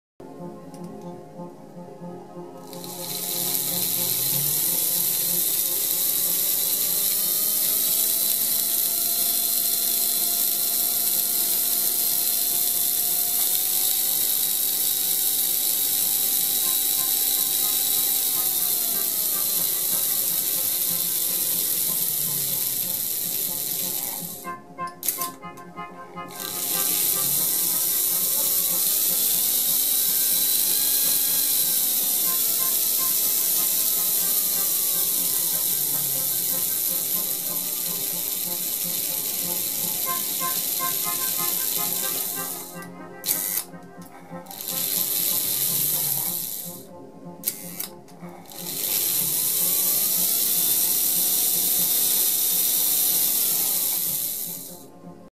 Marklin 3066 version 1
First version of Marklin 3066 reproduction of the prototype 204008 of SNCB. Perfect switch and runs very well.